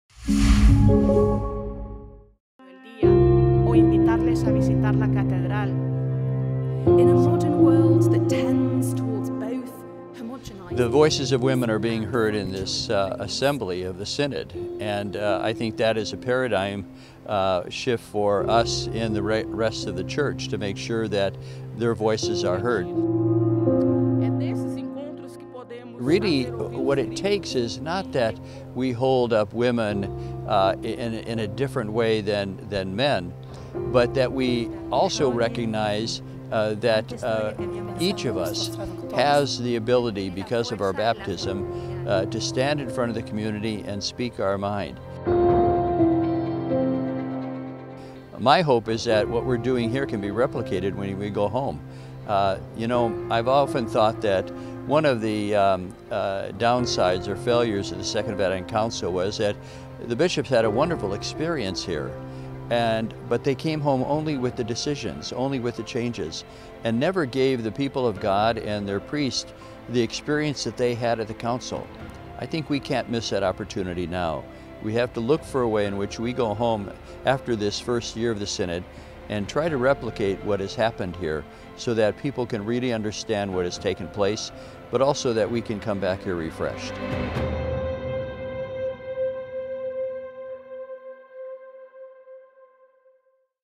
Cardinal: Synod a "paradigm shift" on women
Cardinal Cupich said the synod on synodality is advancing the place of women in the church.